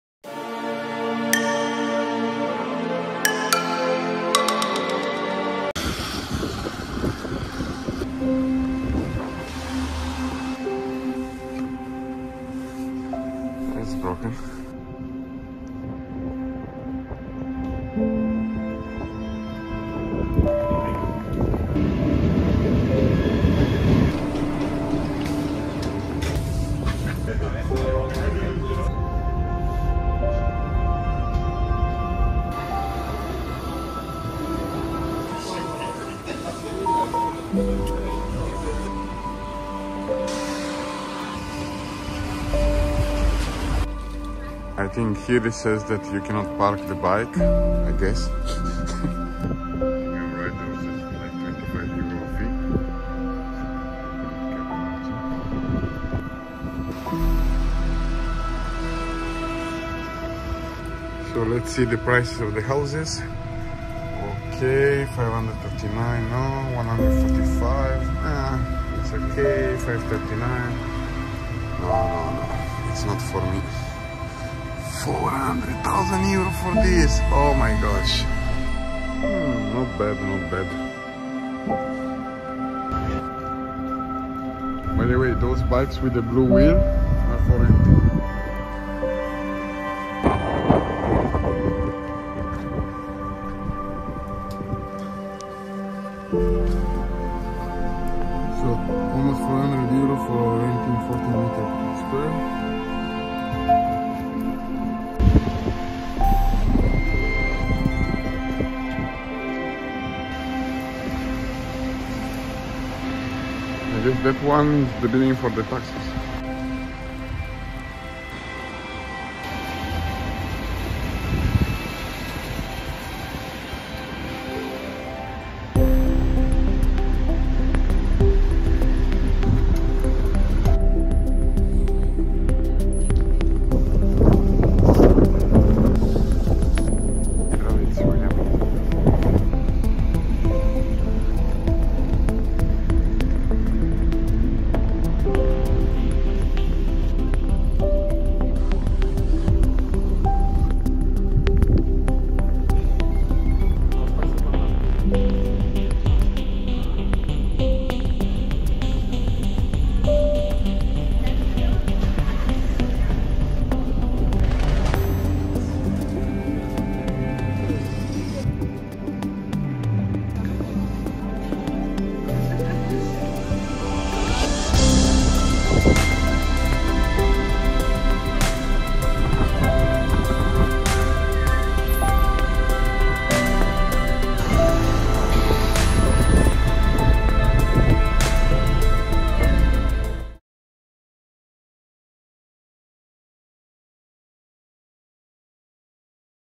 Maastricht 2023, almost 400 euros for renting 14 msq studio
Just a short trip to this beautiful city, one of my favorite in Netherlands